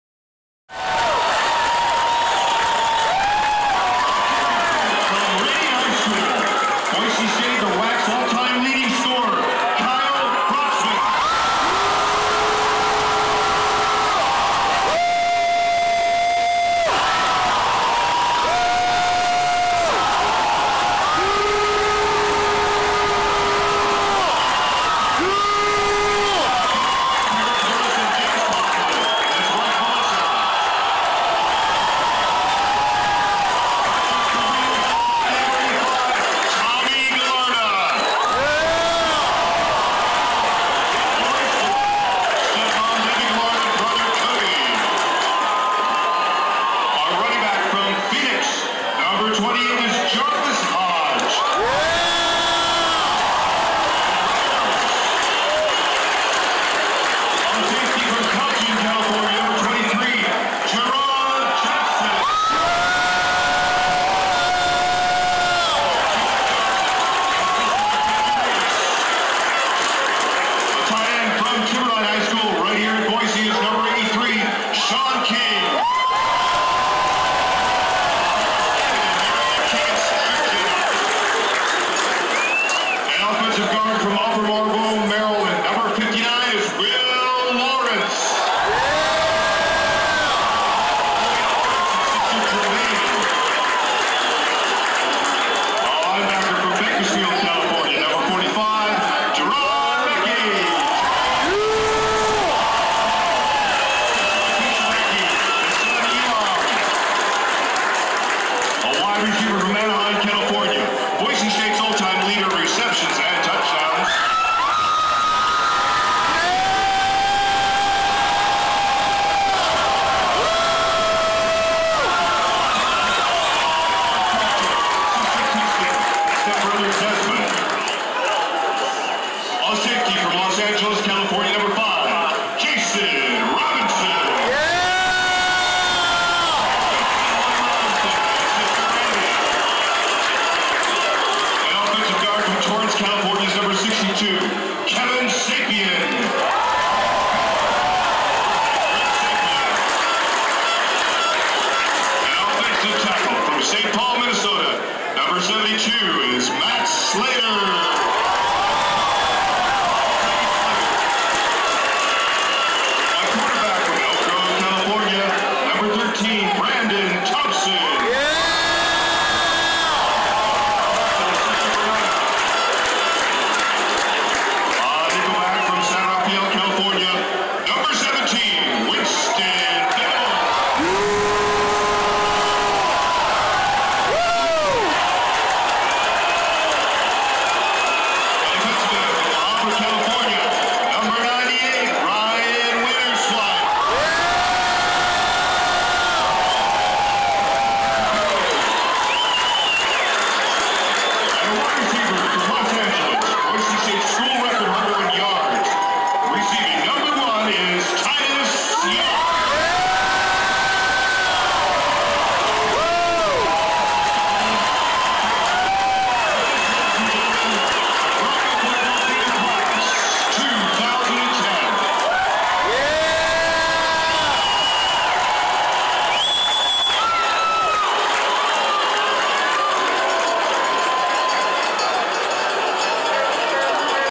Boise State Broncos football senior day 2010
The boise state seniors take the field including kicker kyle brotzman 1 week after missing that huge kick vs nevada